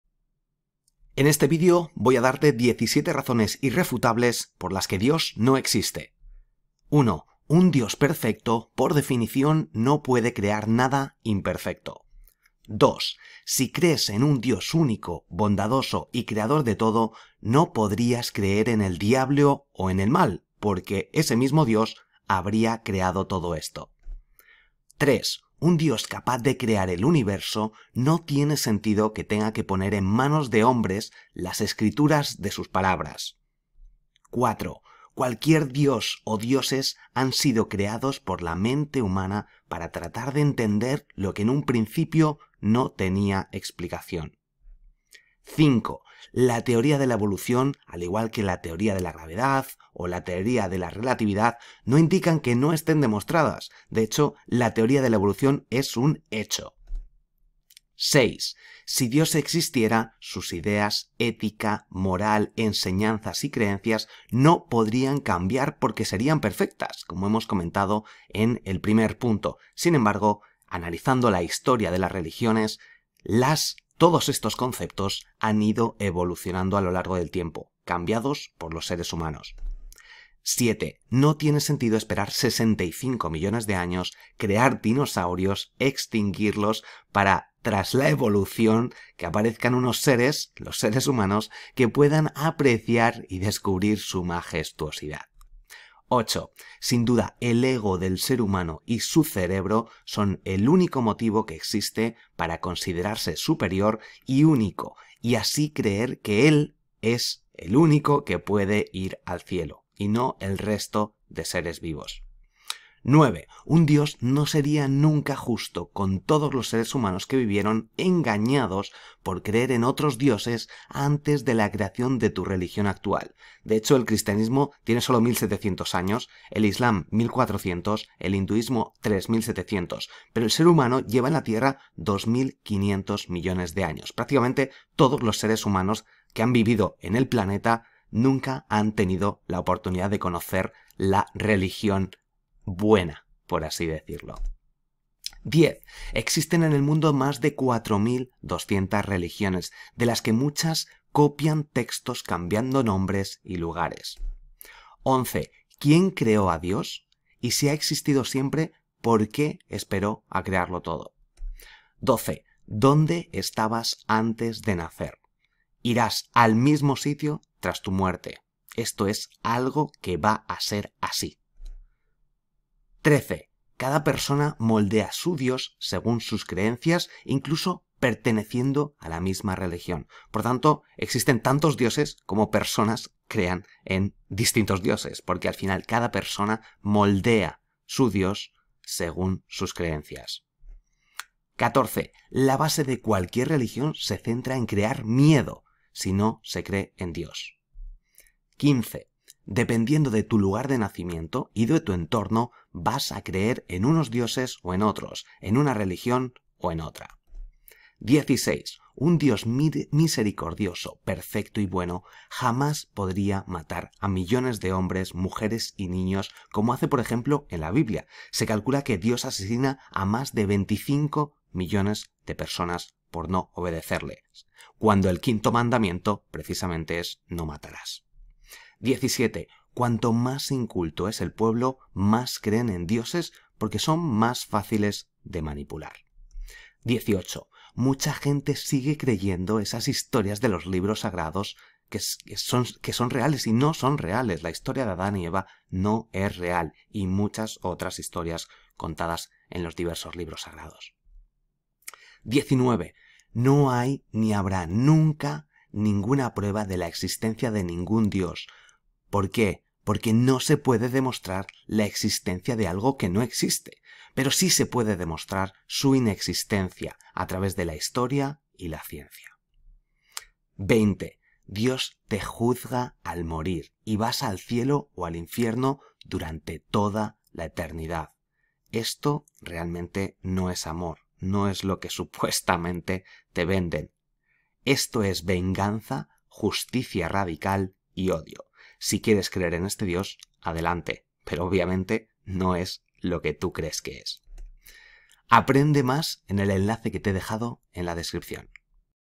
DIOS NO EXISTE: +17 razones irrefutables
Te revelo +17 razones irrefutables por las que Dios no existe.